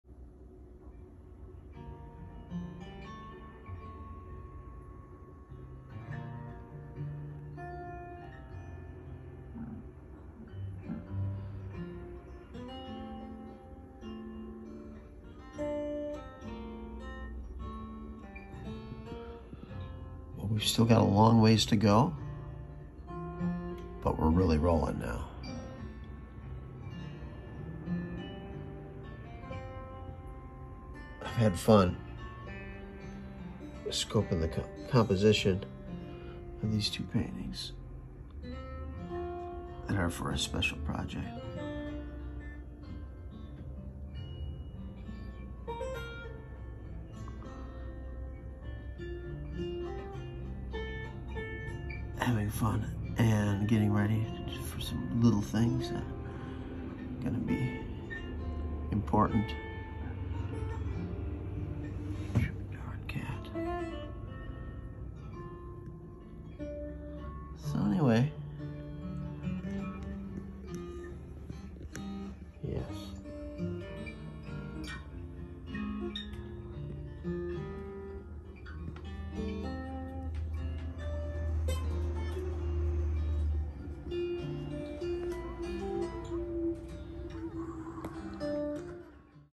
Adventures In The Artbox
Jerpoet shares an update on his current project in today’s Adventure’s In The Artbox with Jerpoet Art